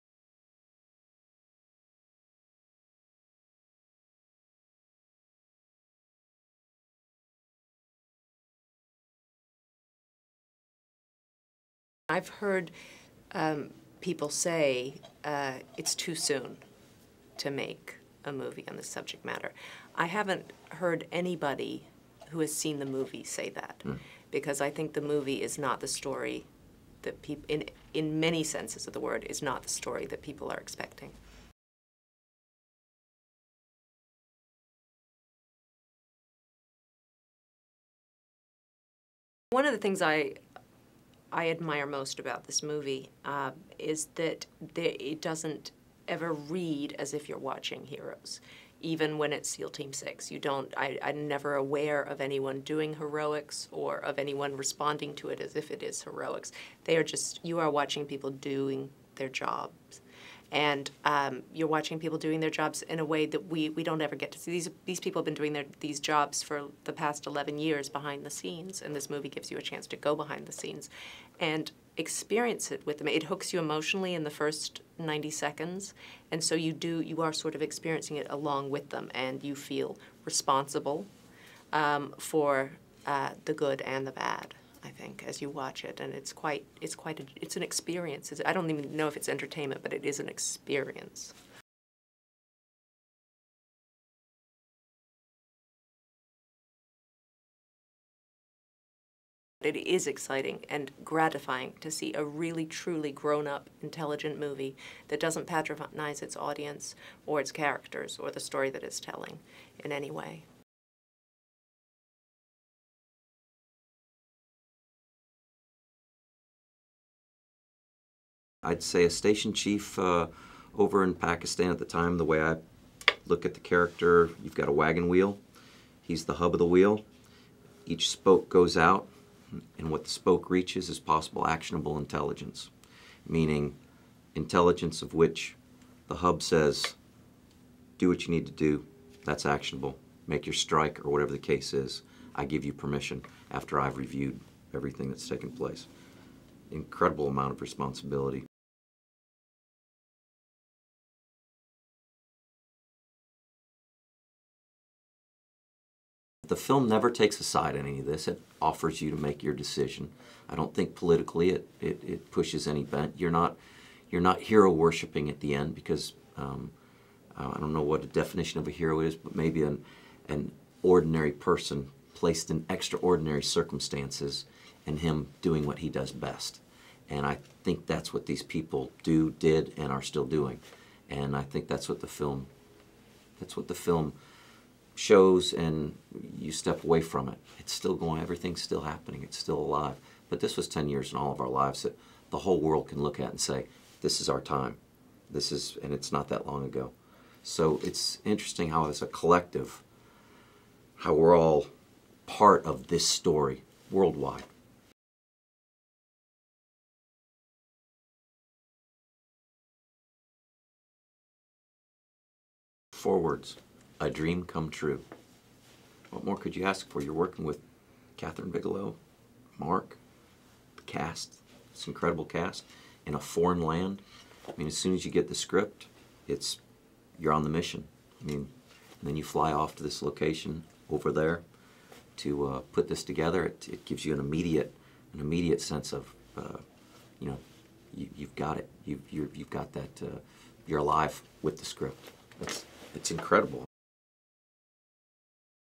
Jennifer Ehle and Kyle Chandler's Official 'Zero Dark Thirty' Interview
After picking up two quick awards in succession, "Zero Dark Thirty" from "Hurt Locker" director Kathryn Bigelow (the only female director to have ever won the Best Director Oscar) has firmed as a favorite to take home some of this year's biggest awards. Headlining that potential haul would be Jessica Chastain, the hard-working actress who has recently been seen in what feels like everything, and who seems set to take her first Oscar statuette on her road to Streep-dom.